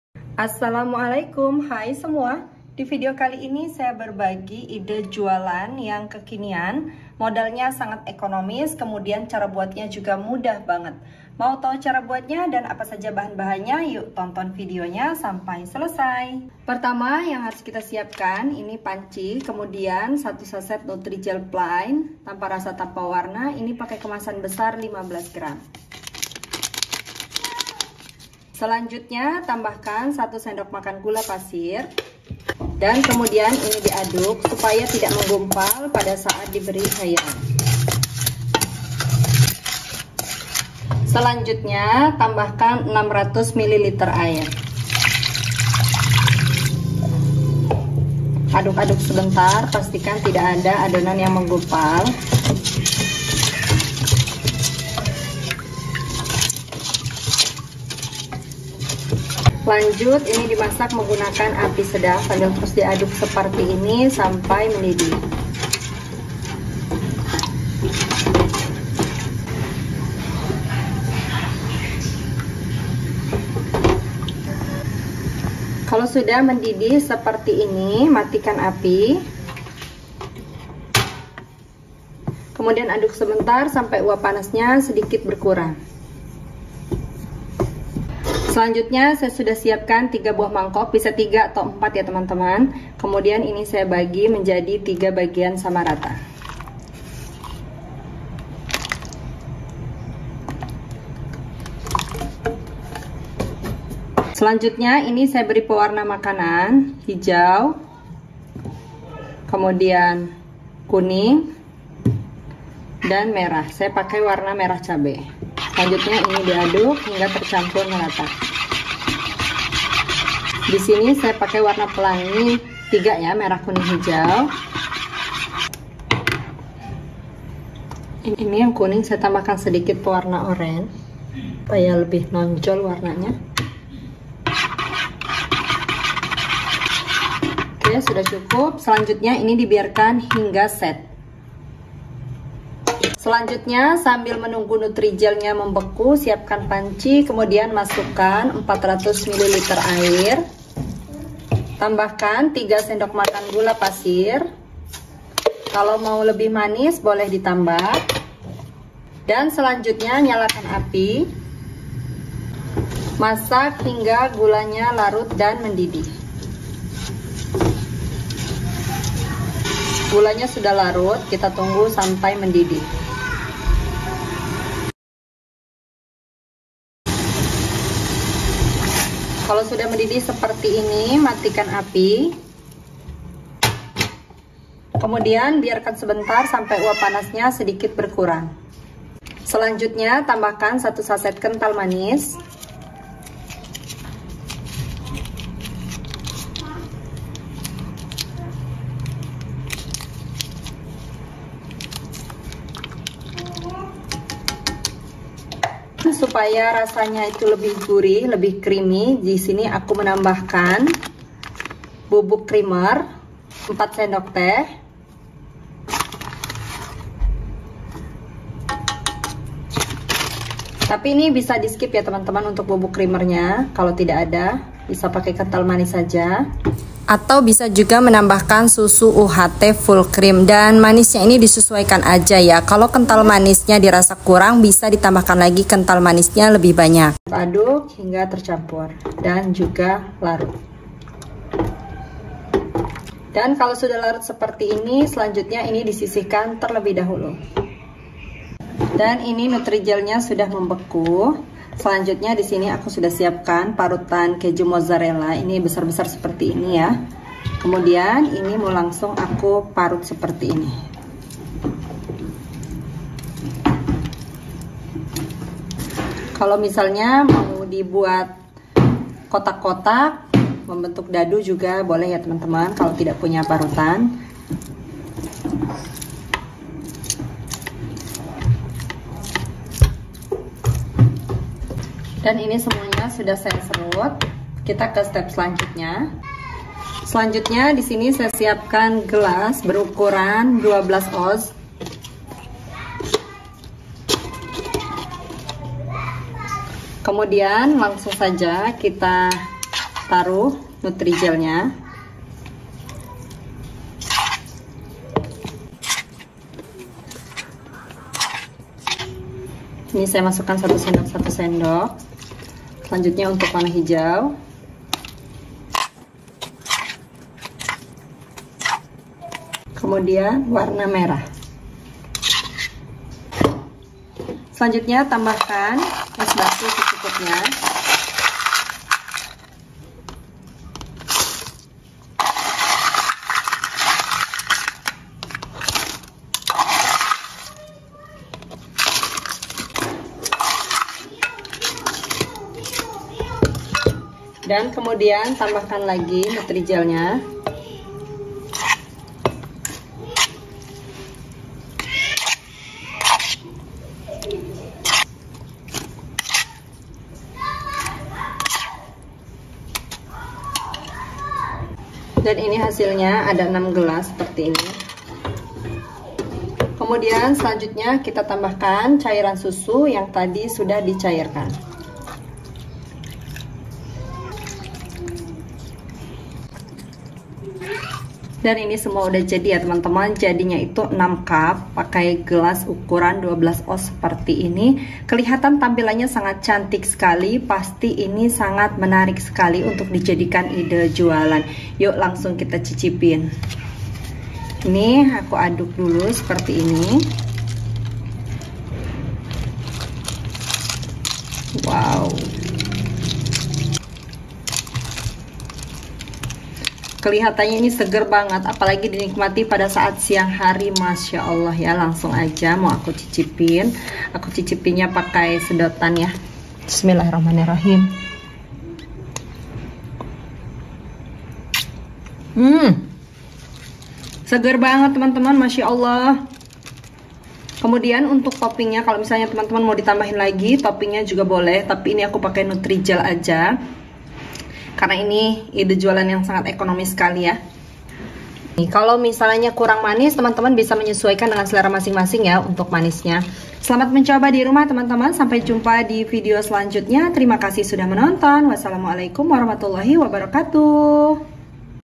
Simpel dan seger banget diminum dimusim Panas ‼️ Es Jelly Pelangi
Bahan-bahan
1 sachet nutrijell plain Rp.5000,-
1 sdm gula pasir Rp.250,-
600 ml air
400 ml air
3 sdm gula pasir Tp.750,-
1 sachet kental manis (kalao maau lebih kental pakai 3 sachet) Rp.2000,-
4-6 sdt bubuk krimer Rp.1000,-